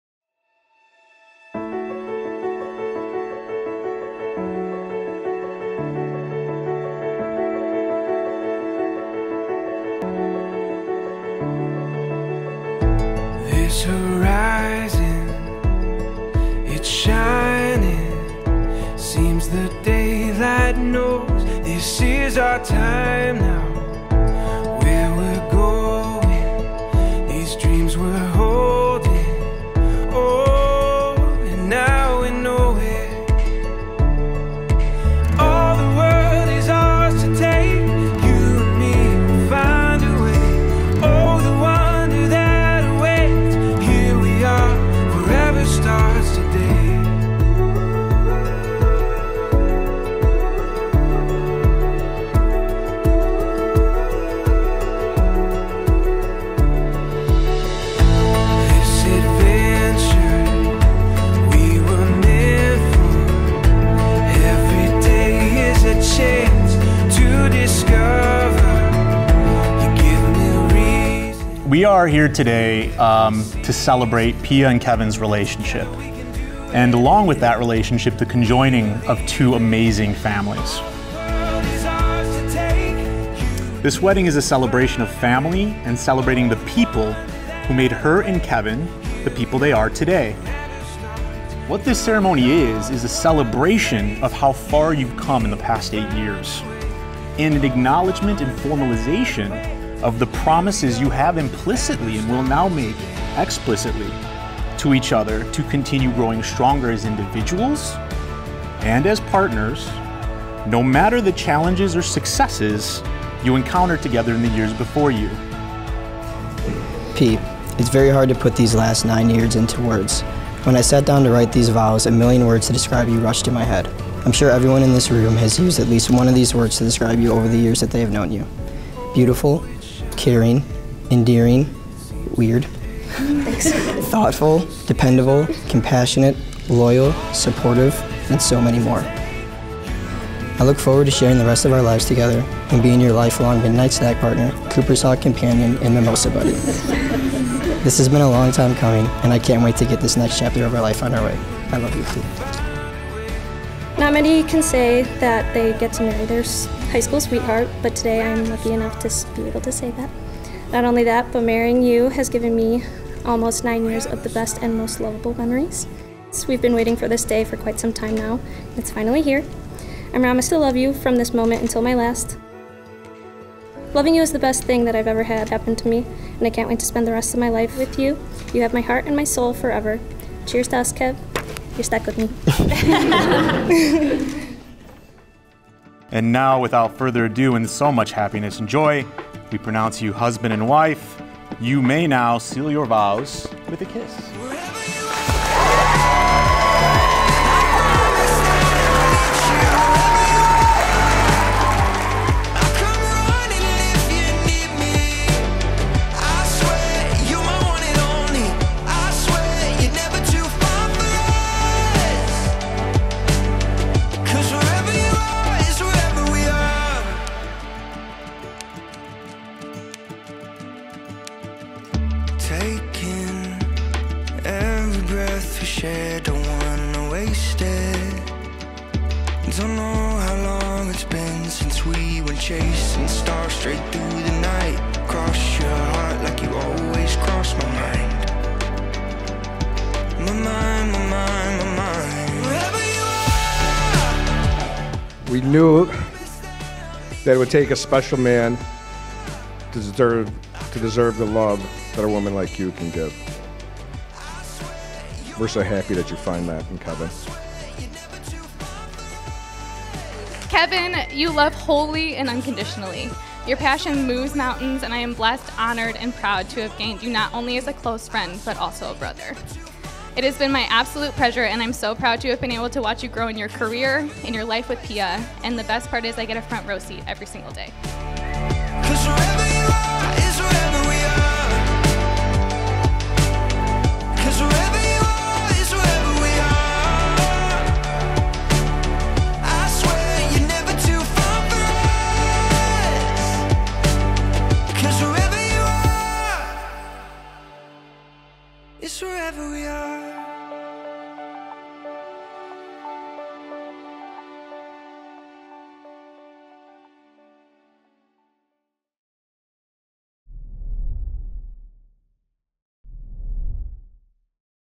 Wedding Video | Olimpia + Kevin | Lombard, Illinois
Wedding Ceremony Venue: Harry Caray Banquets in Lombard, Lombard, Illinois
Wedding Reception Venue: Harry Caray Banquets in Lombard, Lombard, Illinois
Wedding Photography: Henington Studios
Wedding Planning: Wedicity Wedding Day Detailing
Wedding Sound: Spinnin' Discs